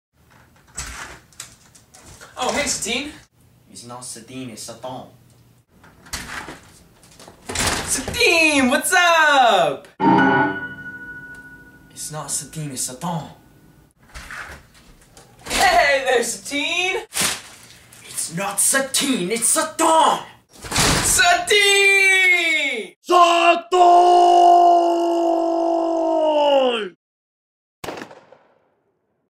SuttOn
Sean has never known how to pronounce my name. He learns his lesson.